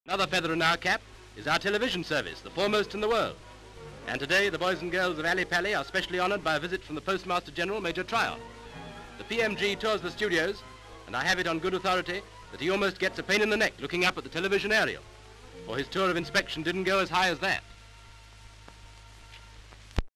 Postmaster General Visits Tv Studios (1938)
Alexandra Palace, London.

Various shots of the Postmaster General Major Tryon arriving at Alexandra Palace television studios.  Good shot of a man recording the arrival with a television camera.  M/S tilts up to show the famous television aerial above the studios.
 FILM ID:957.07